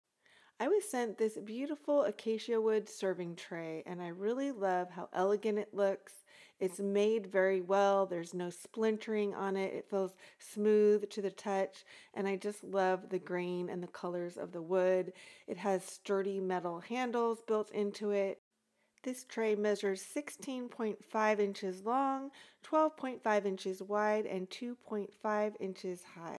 Serve in Style: Comfify Acacia Wood Serving Tray Review! 🌟
Hey there, foodies and entertainers! Today, we're taking a closer look at the Comfify Acacia Wood Serving Tray – the ultimate accessory to elevate your dining experience.

Picture this: presenting your favorite dishes on a beautifully crafted serving tray that not only enhances the visual appeal but also adds a touch of sophistication to any occasion. That's exactly what you get with the Comfify serving tray.

Here's why it's a must-have for every home:

Exquisite Craftsmanship: Made from premium acacia wood, this serving tray exudes elegance and charm. With its oil brown finish and sleek black handles, it's a stunning addition to any table setting.

Splinter-Free Guarantee: Say goodbye to worries about splintering! Crafted with care, this tray boasts a smooth surface that's gentle on your hands and safe for serving food.

Versatile Functionality: Whether it's breakfast in bed, a cozy dinner on the couch, or entertaining guests on the patio, this tray is your go-to companion. Its versatility knows no bounds!

Easy Handling: Thanks to its ergonomic grip handles, carrying this tray from the kitchen to any part of your home is a breeze. Plus, its lightweight design ensures comfortable transportation, even when it's loaded with delicious treats.

Built to Last: Designed for daily use and heavy-duty tasks, this tray is a long-lasting investment in your dining and entertaining essentials. It's the perfect blend of style and durability.

Upgrade your dining style and make every meal a memorable experience with the Comfify Acacia Wood Serving Tray.  🌟